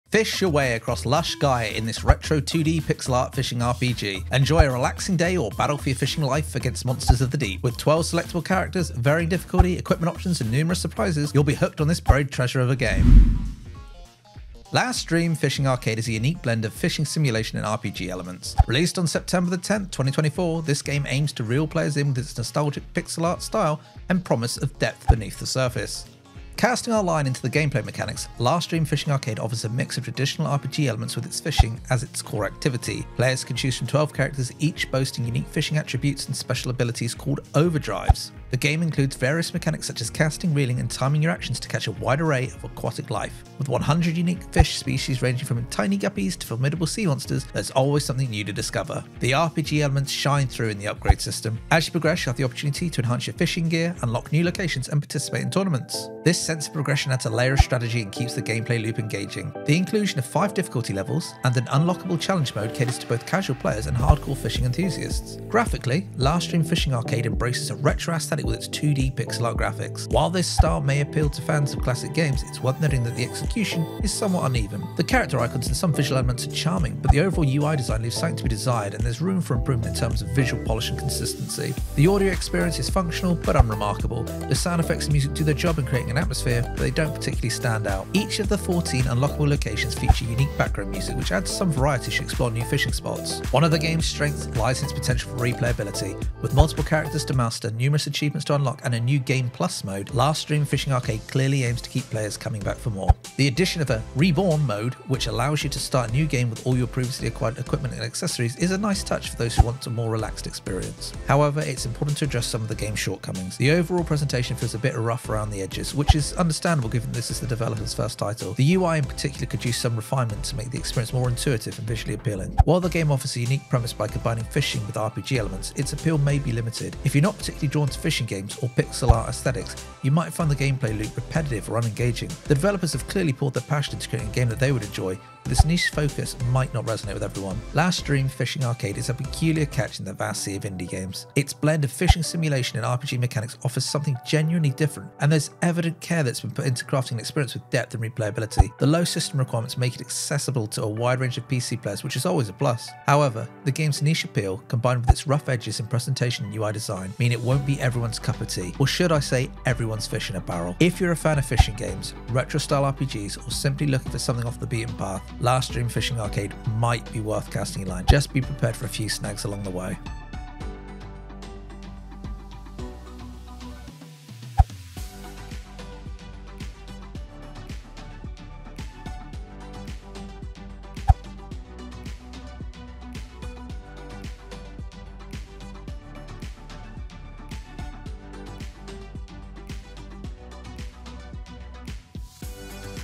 Look, A Fishing RPG!
Dive into the pixelated world of Last Dream Fishing Arcade, the quirky fishing RPG that's making waves! Released on September 10, 2024, this indie gem offers a unique blend of retro charm and modern gameplay mechanics.

🎣 Experience nostalgic 2D pixel-art graphics
🎮 Master 12 unique characters with special abilities
🌊 Explore 14 diverse fishing locations
🐠 Catch 100 different fish species, from guppies to sea monsters
🏆 Compete in tournaments to become the Fishing God

Will you get hooked on Last Dream Fishing Arcade, or throw this one back?
Our comprehensive review covers all the bait and tackle:
✅ Engaging RPG progression system
✅ High replayability with multiple game modes
✅ Low PC requirements for accessibility
❌ Uneven UI design
❌ Basic sound and music

Is Last Dream Fishing Arcade the catch of the day or just another fish in the sea? Watch our full review to find out if this fishy adventure is worth your time and money!